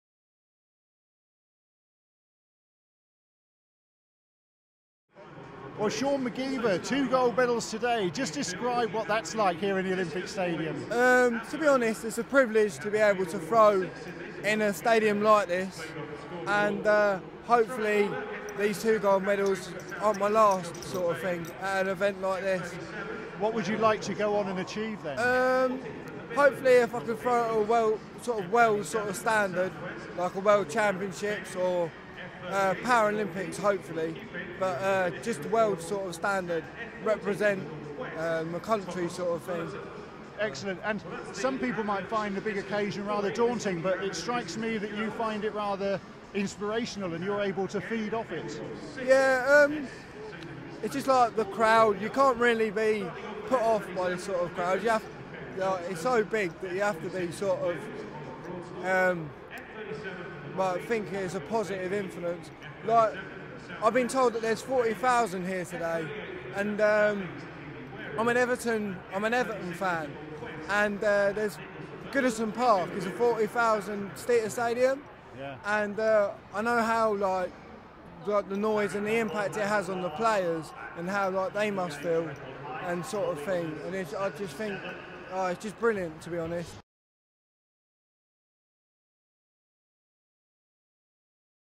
Sean McGeever: Sainsbury's 2012 School Games, Ambulant Shot Put Gold Medallist
Double gold medallist, Sean McGeever, speaks to Media First about winning at the Sainsbury's 2012 School Games in the Olympic Stadium, comparing his experience to Everton playing at Goodison Park, and sharing his aspirations for the future.